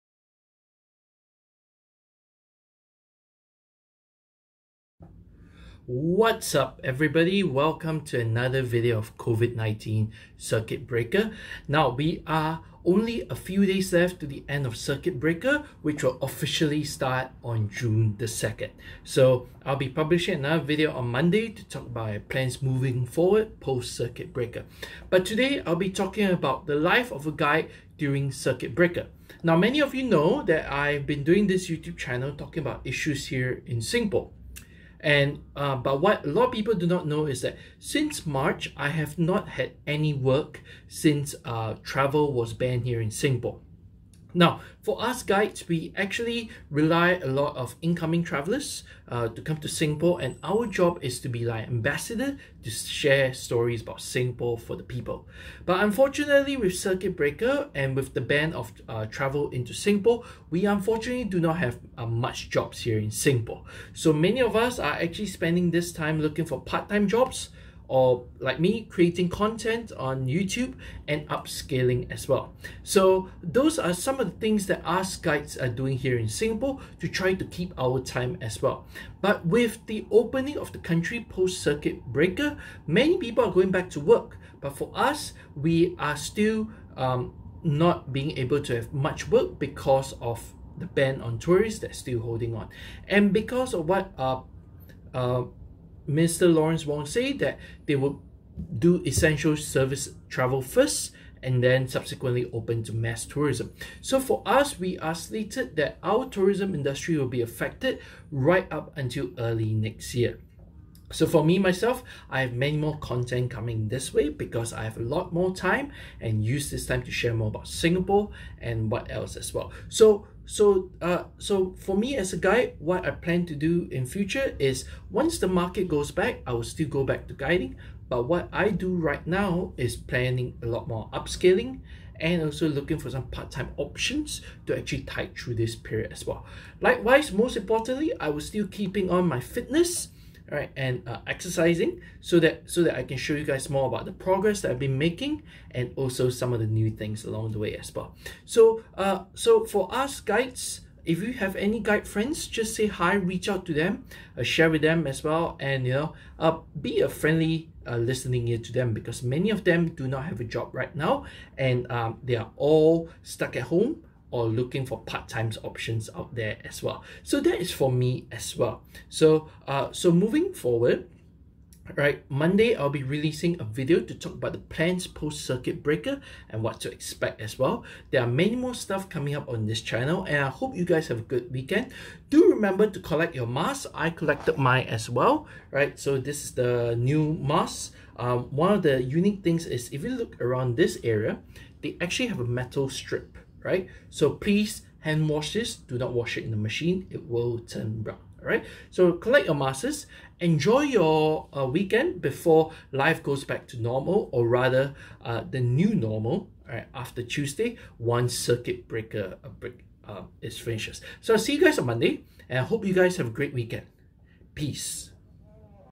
Circuit Breaker:Life of a tourist Guide
Life of a Tourist Guide during Circuit breaker. Not really a lot of work